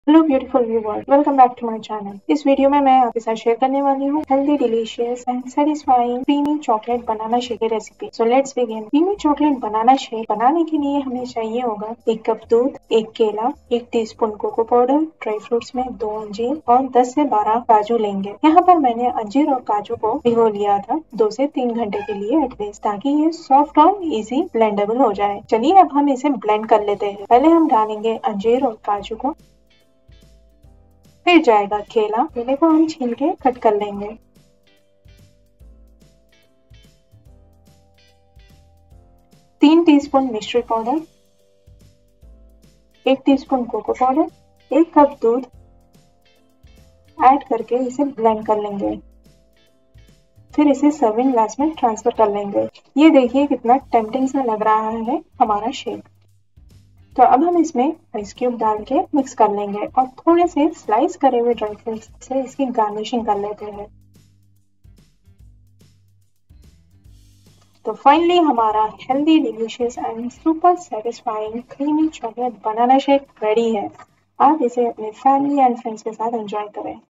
Creamy Chocolate Banana Shake Recipe - Refreshing & Appetizing | Summer Drinks
Ingredients -

Full cream milk - 1 cup
Banana - 1
Cocoa powder - 1 tsp
Dry fig - 2
Cashew nuts - 12
Rock sugar powder - 3 tsp
Sliced dry fruits for garnishing

By doing so, I can bring you a variety of interesting recipes and videos 😊

2️⃣ Please kindly share the recipe with your beloved ones.

3️⃣ Give a huge thumbs up 👍 to show your support!

4️⃣ Thanks a lot...stay blessed ❤️😇
Related keywords -

YT Cooking Official